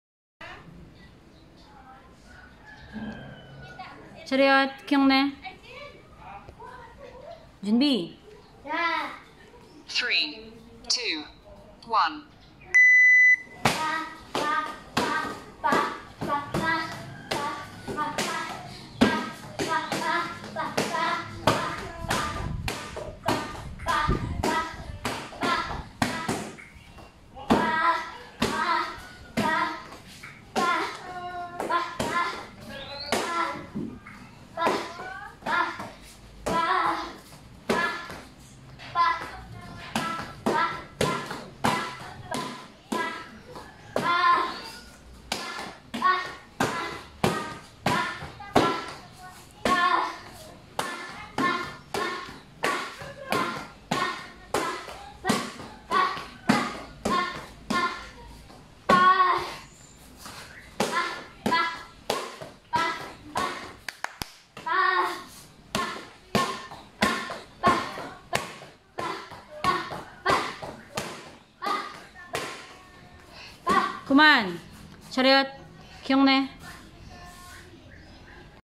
DELLO HANNAH MARGARETTE , CENTRAL PHILIPPINE UNIVERSITY - ILOILO, KICKPAD
2020 SMART/MVPSF NATIONAL TAEKWONDO INTERSCHOOL SPEED KICKING CHAMPIONSHIP